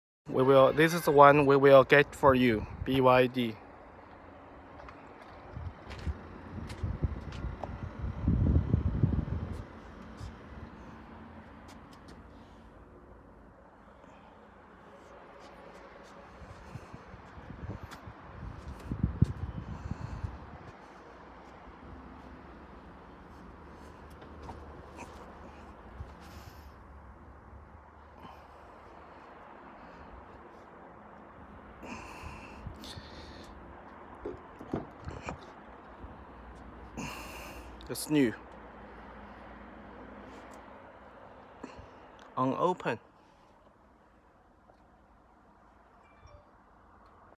massive stockable BYD cars/BYD song plug flagship with competitive price
BYD Song Plus EV is a mid-range budget-friendly electric SUV launched in April 2021.
It supports up to 90 kW DC charging. Song Plus EV can be fully charged in exactly 74 minutes.
Song Plus EV uses BYD's proprietary LFP battery cell design called 'Blade Battery'. It offers more than 500 kilometers of range when fully charged.